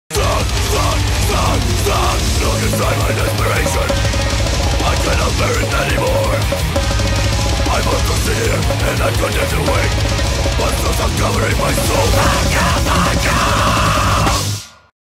BLASTCORE tour 2016 - Praha 21.4. - pozvánka
Ve čtvrtek 21.4. proběhne v Modré Vopici extrémní death grindově alternativní večírek! PERFECITIZEN, PSYCHOTIC DESPAIR, FLESHSOUL! Přijď podpořit start BLASTCOREtour 2016! Facebook event